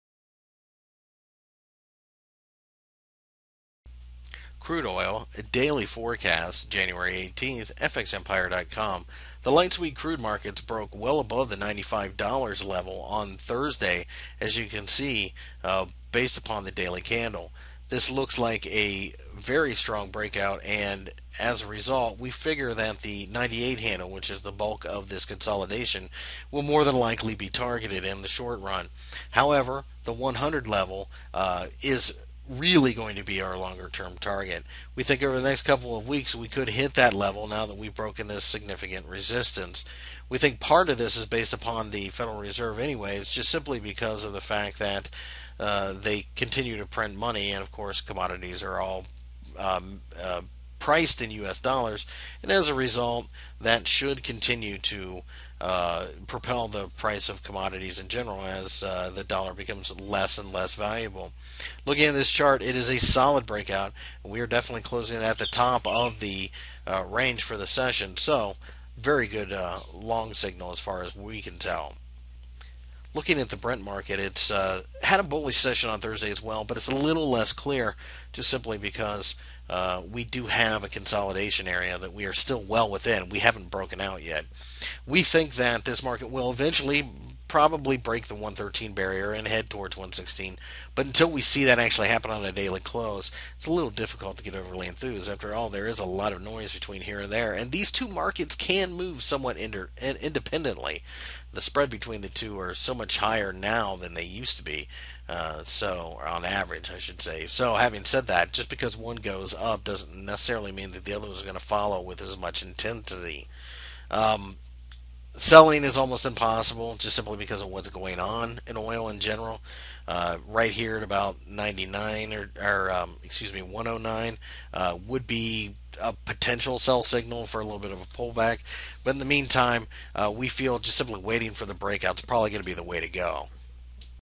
Oil Technical Analysis for January 18, 2013 by FXEmpire.com
- January 18, 2013 commodity daily technical analysis for the Oil commodity. Find more information about Forex and Commodities News